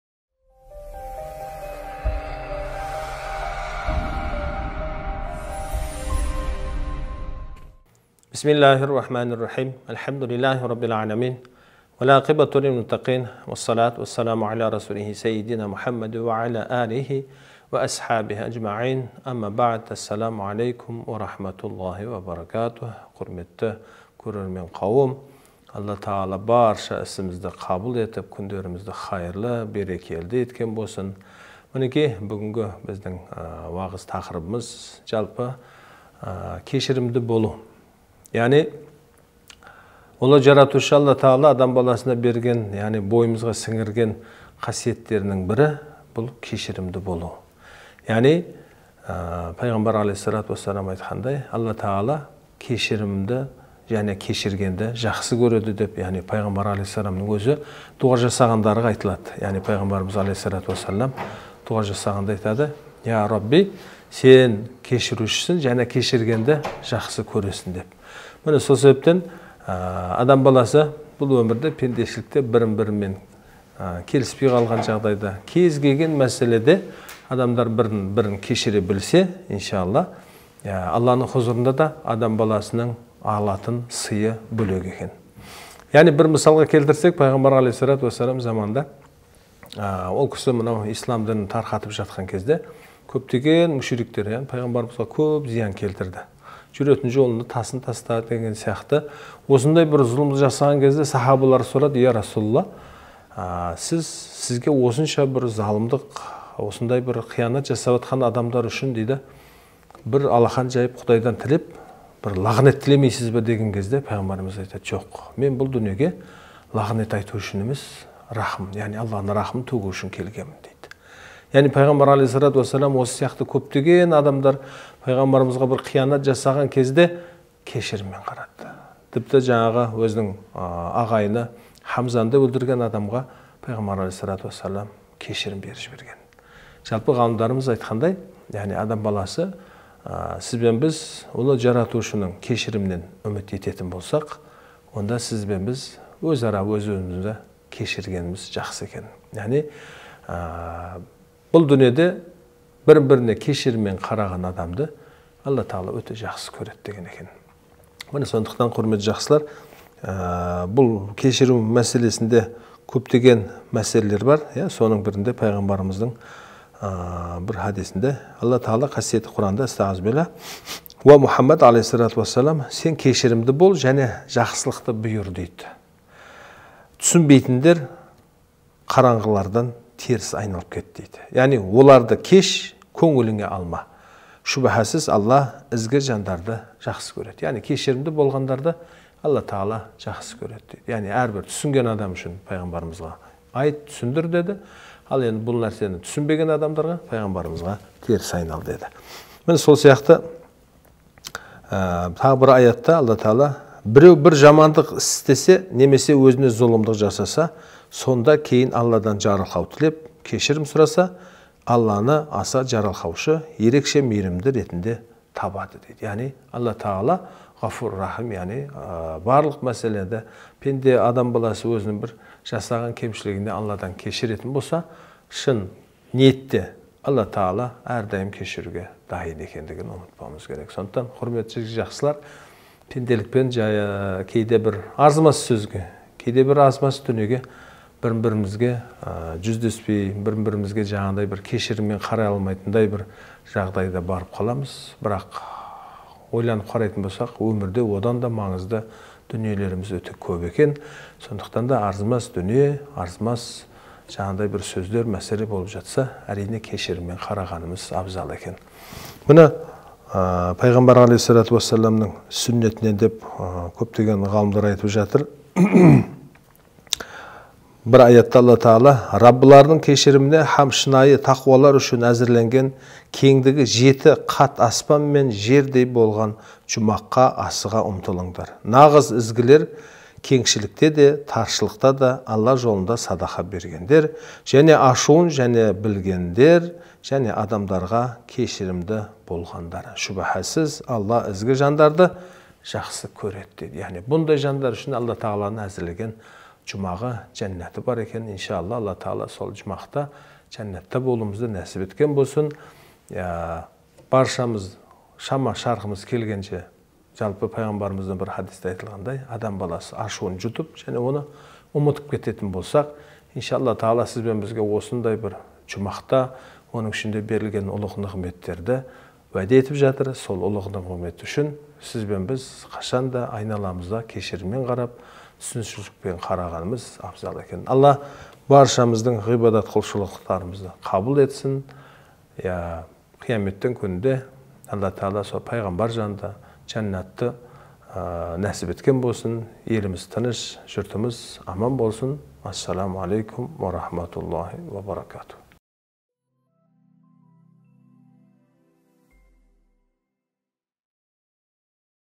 "КЕШІРІМДІ БОЛУ – КЕҢШІЛІК" Мұрат Тастанбеков
"БОЙЫТ АТА" мешітінің имамы